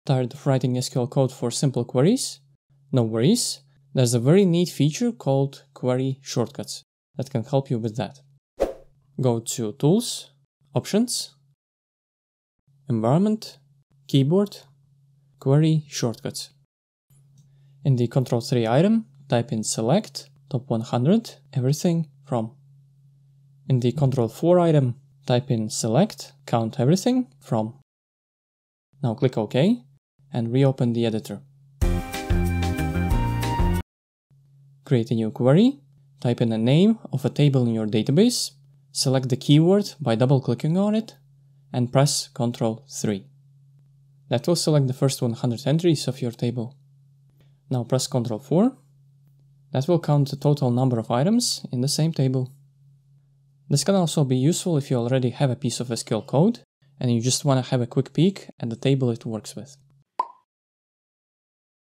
How to Add a Query shortcut in SQL Management Studio
Ever needed to quickly view a few items on the table or call some other simple query? There's a very neat feature in SQL Server Management Studio called Query Shortcuts. Essentially you can save a query and apply it on a table of your choice using a shortcut.